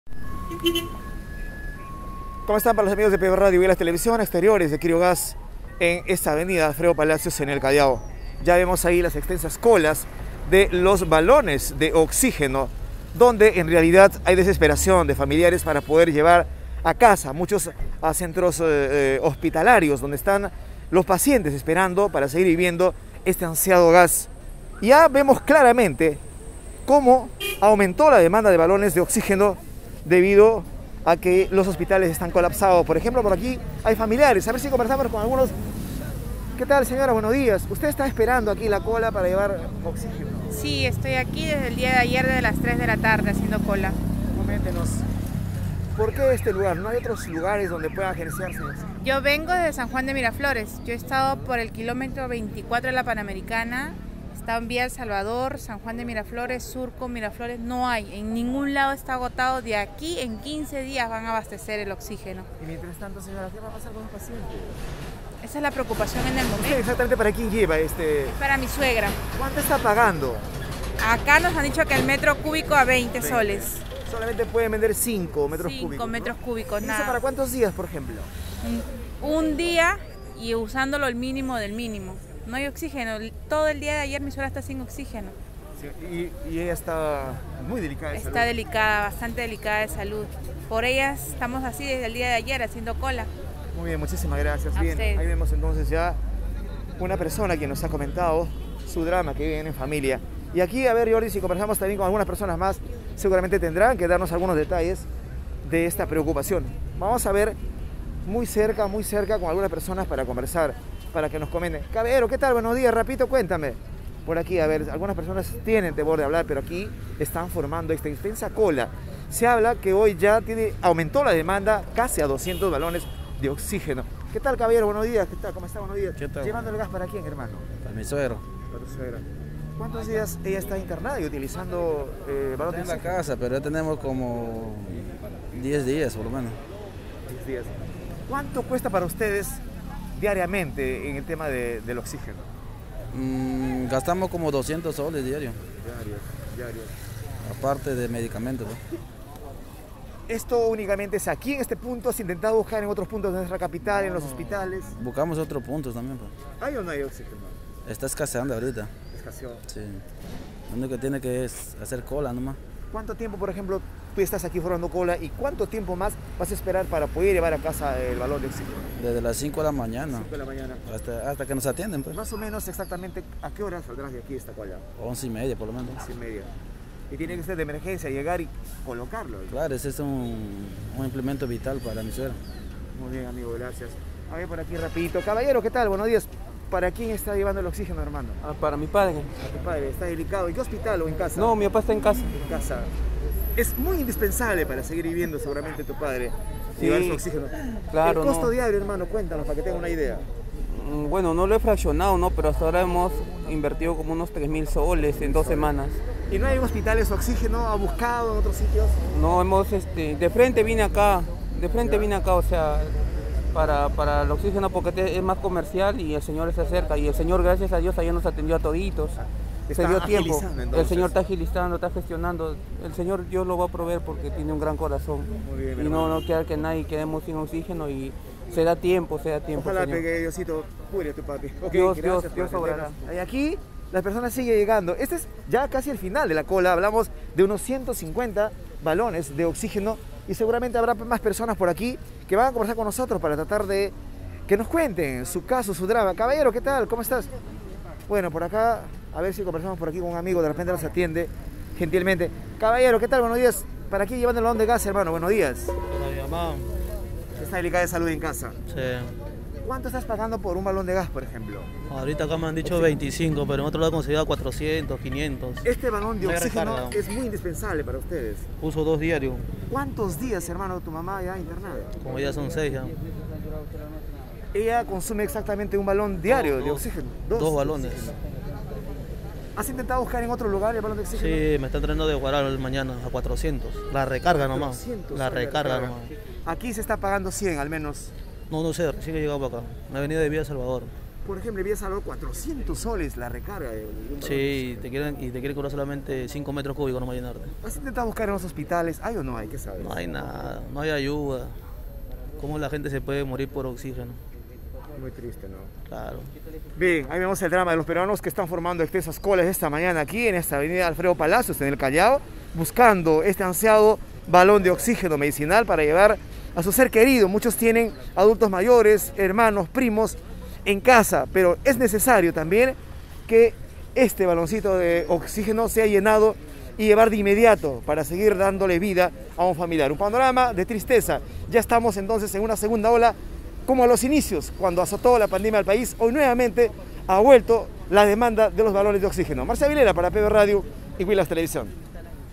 EN VIVO Decenas de personas pugnan por adquirir oxígeno: "Está agotado, esa es la preocupación"
👉 ENVIVO AHORA desde

🔴 Decenas de personas pugnan por adquirir oxígeno: "Está agotado, esa es la preocupación"

📻 PBORadio 91.9 FM ¡LA RADIO CON FE!